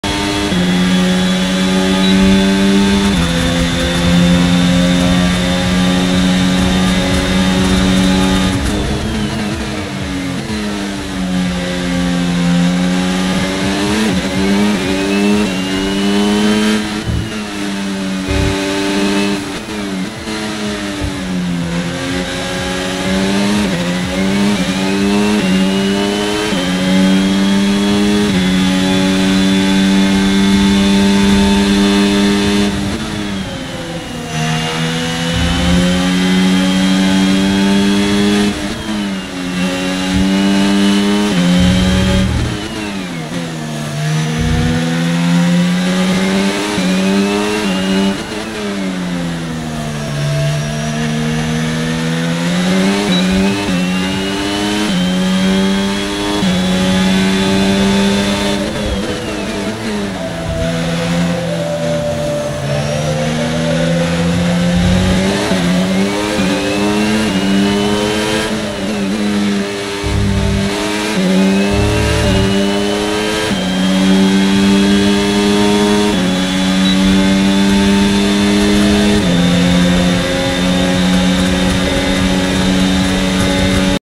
F1 Holanda
Uma volta no gp da Holanda com a carro da Red Bull.